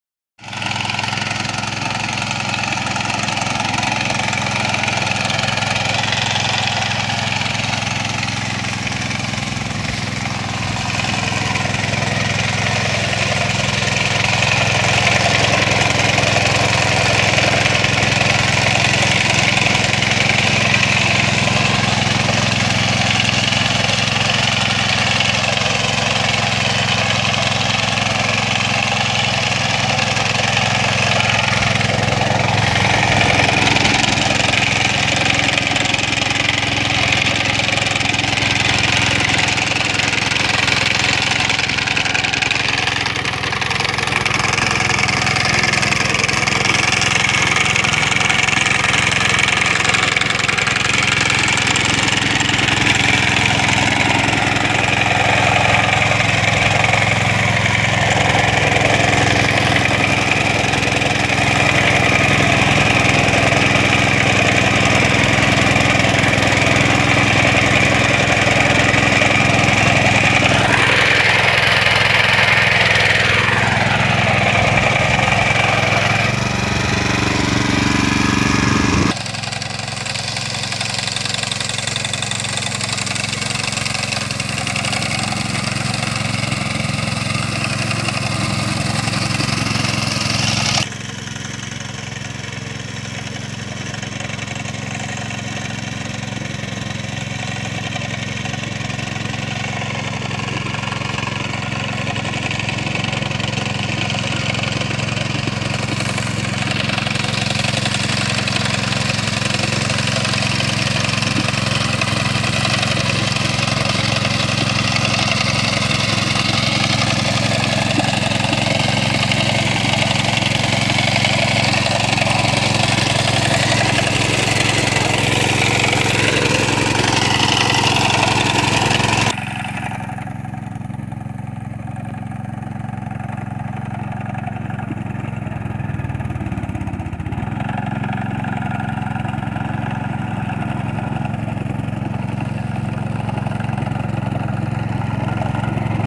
Cotumda moto cositoare ruris
Cum sa citesti cu ruris cea mai buna marca de cositoare din romania e foarte usoara de condus